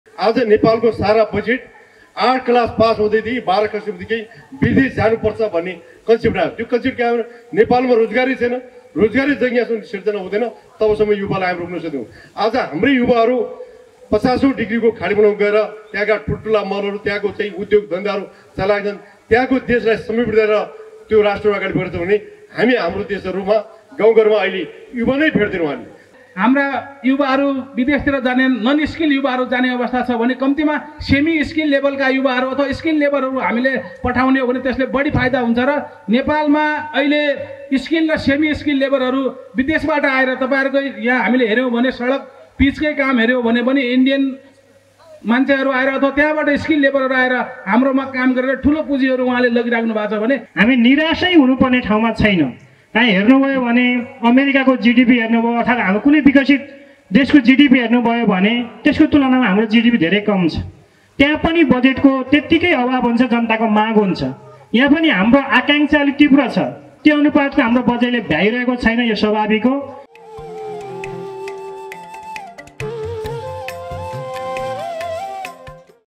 जनतामा आकांक्षा तिब्र छ तर निरास नै हुनुपर्ने अवस्था छैन, परशुराम साहित्य महोत्सव | Parshuram Dham
Parshuram Dham
जनतामा आकांक्षा तिब्र छ तर निरास नै हुनुपर्ने अवस्था छैन, परशुराम साहित्य महोत्सव विशेष आज राति ८:०० बजे !

परशराम साहित्य महोत्सव - २०८१
  पहाडी रानी डडेल्धुरा: सम्भावनाको खानी परशुराम
भीम साउद कांग्रेस कांग्रेस जिल्ला सभापति
एमाले जिल्ला अध्यक्ष
माओबादी जिल्ला अध्यक्ष

@by Yatri Media network Pvt.Ltd